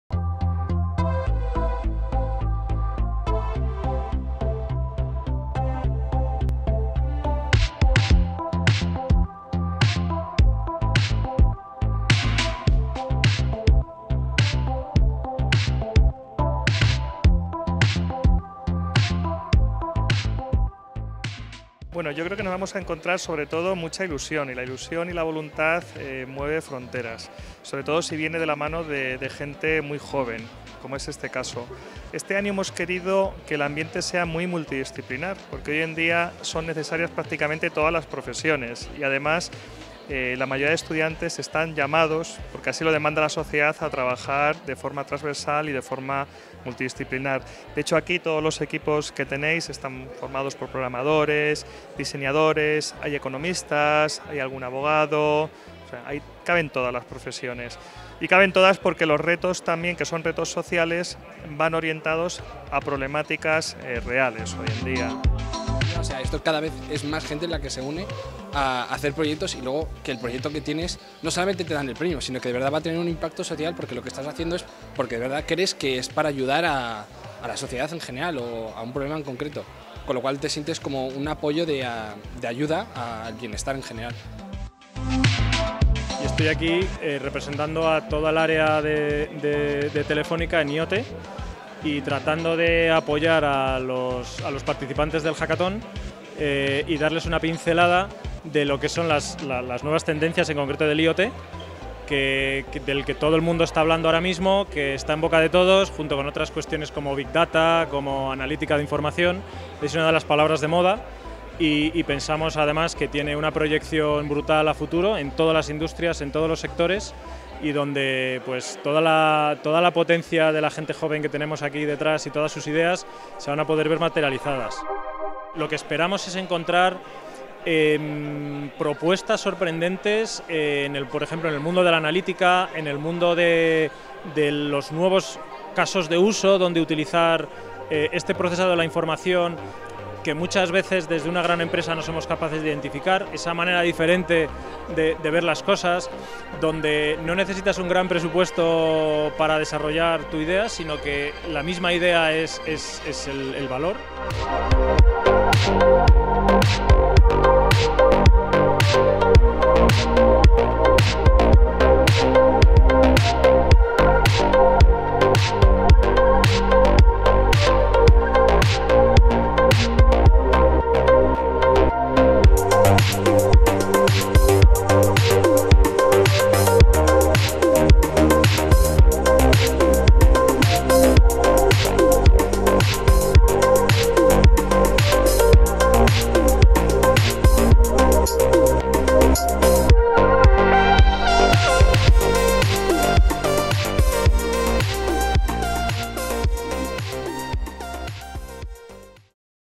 Trece premios por hackear ‘for good’ la sociedad | ESNE
6 premios Think Big H4G, 2º Premio Universidad Rey Juan Carlos, 3º Premio UNIR, 1º Premio ‘Mejor Serious Game’ Universidad Complutense, 1º y 3º Premio a ‘Mejor Diseño’ y 2º Premio HackForGood Madrid, suman los trece premios otorgados a equipos formados por alumnos y graduados de ESNE. _HackForGood 2018_